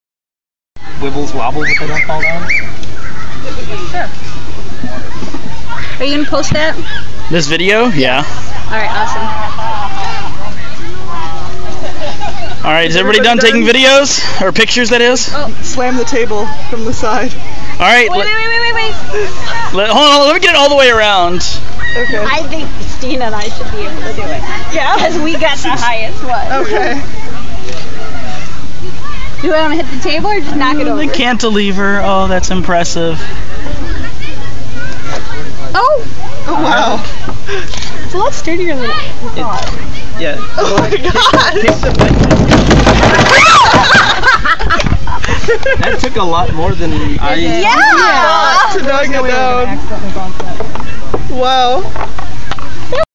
Cornerstone, The Finale
After a rousing game of Cornerstone, which can only end by running out of pieces or knocking the tower down, we found that running out of pieces just wasn't satisfying enough.

Tower, meet gravity.

Filmed with my phone, rotated with Windows Movie Maker